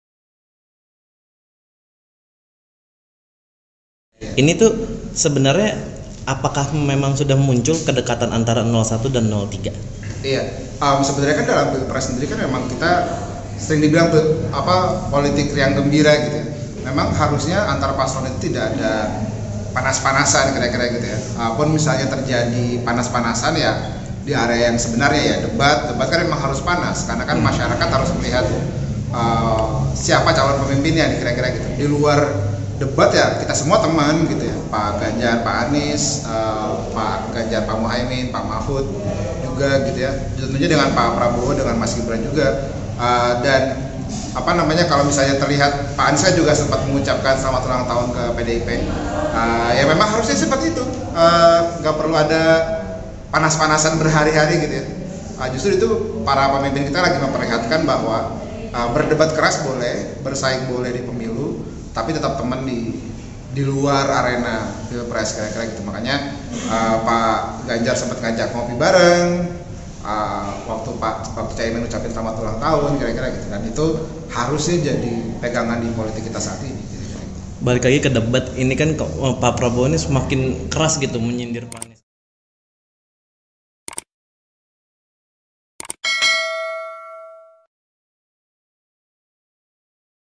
Timnas Amin: Berdebat Keras Boleh tapi Tetap Teman di Luar Arena
Tim Nasional Pemenangan Anies Baswedan - Muhaimin Iskandar (Timnas Amin) menjawab isu kedekatan antara kubu paslon 01 dan 03, Jumat (12/1/2024).

Juru Bicara Timnas Amin Angga Putra Fidrian mengatakan, Anies dan Muhaimin menganggap semua lawannya adalah teman ketika di luar kontestasi politik.

Memang harusnya antar paslon tidak ada panas-panasan, apabila terjadi panas-panasan ya di area yang sebenarnya, ujar Angga saat ditemui di Markas Pemenangan Timnas Amin, Menteng, Jakarta Pusat.

Angga mengatakan, pertemanan itu harusnya menjadi pegangan di kontestasi politik saat ini. Ia pun mencontohkan Anies yang sempat mengucapkan ulang tahun ke PDI-P yang merupakan lawan dalan Pilpres 2024.

Video Jurnalis: Syalutan Ilham
Penulis Naskah: Syalutan Ilham
Video Editor: Syalutan Ilham
Produser: Adil Pradipta